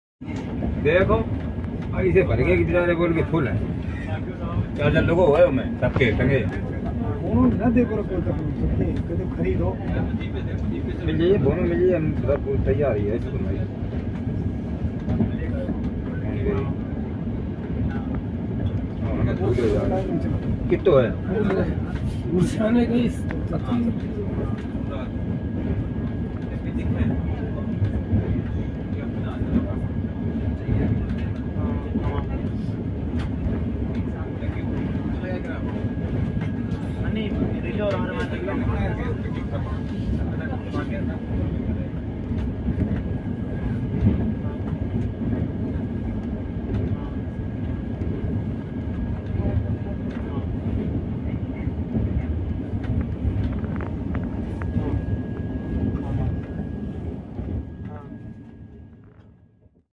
Godavari river crossing by Tamilnadu express
12622 Tamilnadu express crossing Godavari river
indias one of the longest bridge in telangana